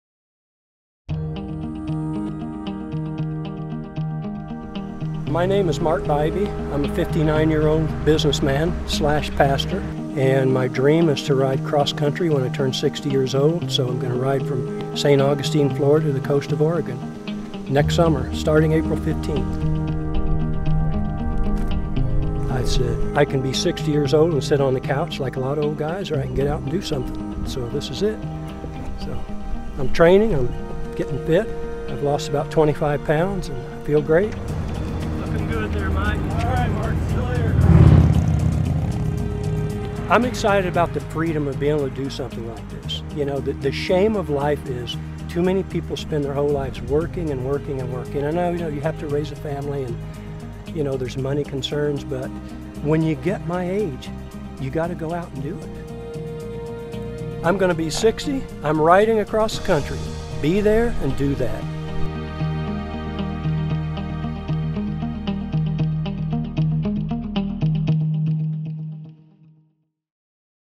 Be There Do That - Riding Strong at 60 - Mark
Mark is going to be 60 and has decided to "get off the couch" and do something incredible. He is going to ride his bike across the country.  We were fortunate to meet him and hear his story as we were traveling with Mike Ehredt of Project America run while on the Nachez Trace.  Mike encouraged Mark to follow his dream and literally "Be There and Do That".  Both men are inspiring stories of doing more and living well.